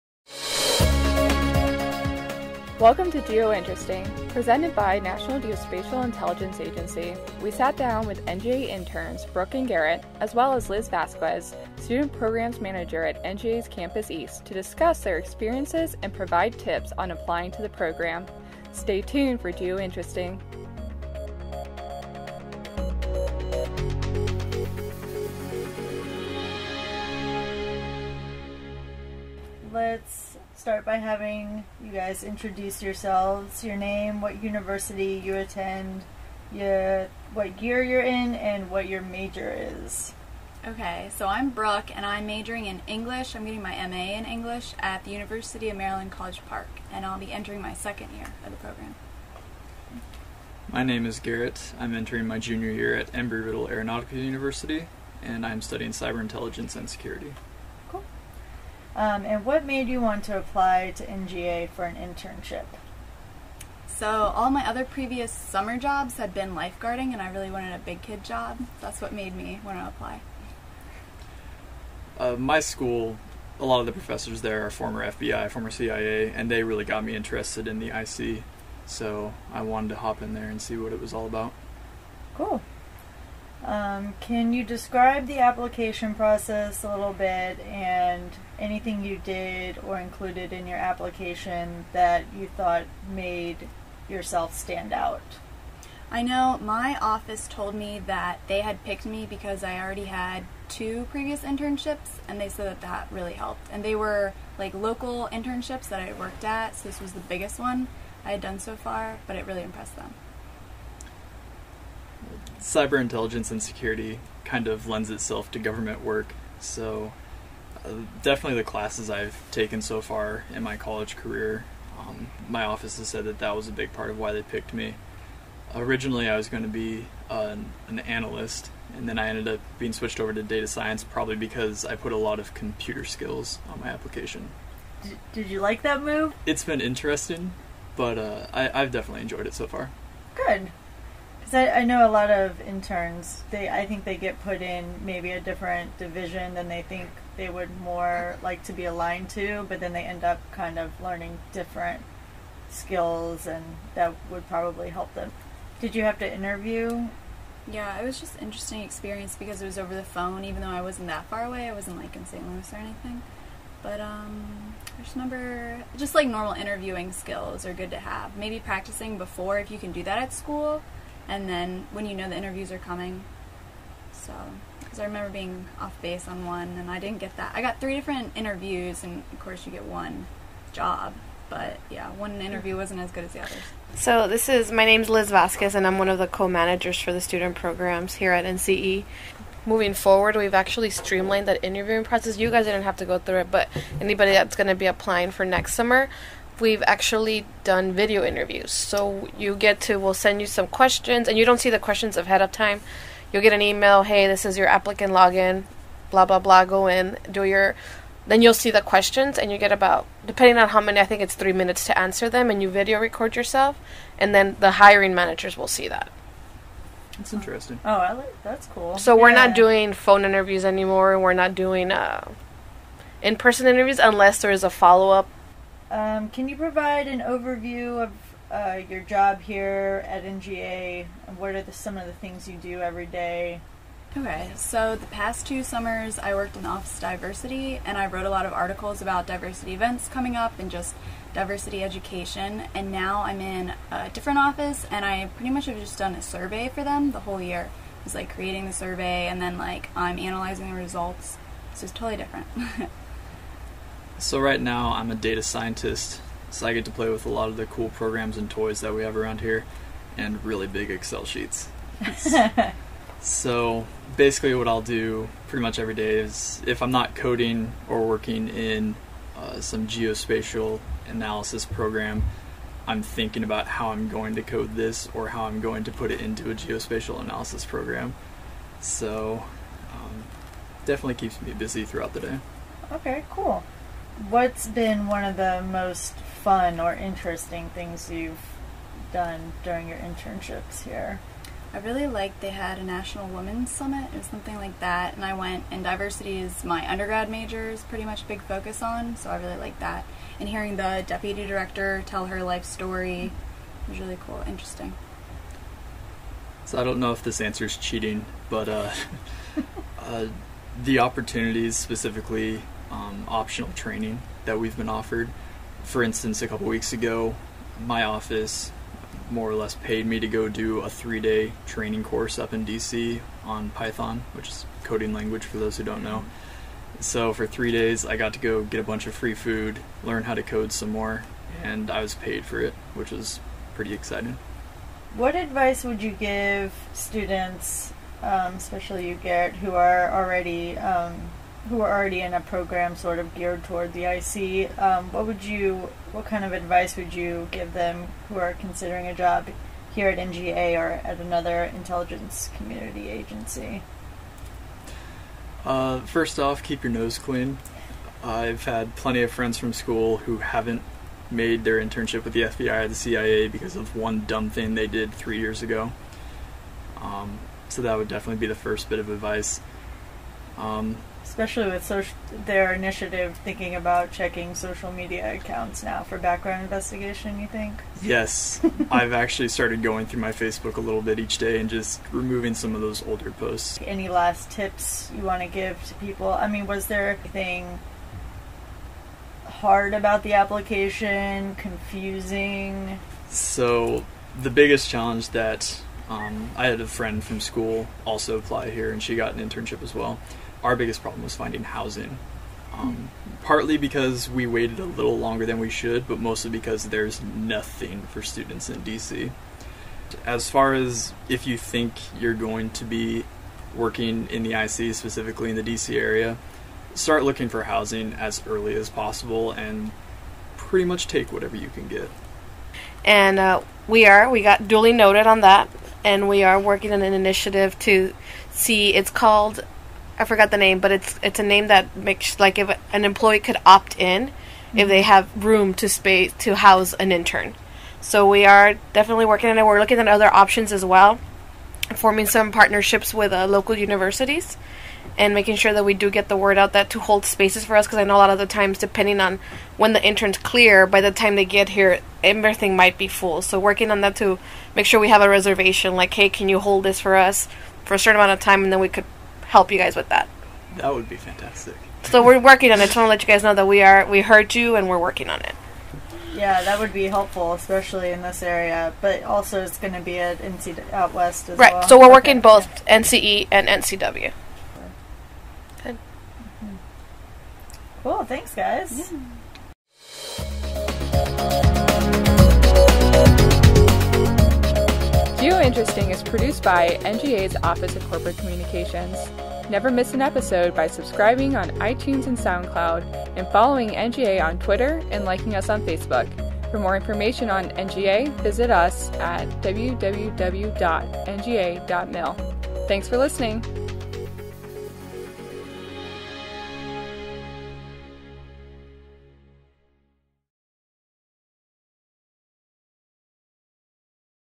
Geointeresting Podcast, Episode 13: What is it like to be an intern at NGA?
Geointeresting sits down with two National Geospatial-Intelligence Agency interns from this past summer to discuss their unique experiences as interns in the intelligence community. They are also joined by one of the agency's student programs managers for some helpful tips to becoming an intern at the agency. Interested in learning more about NGA's internship program or applying?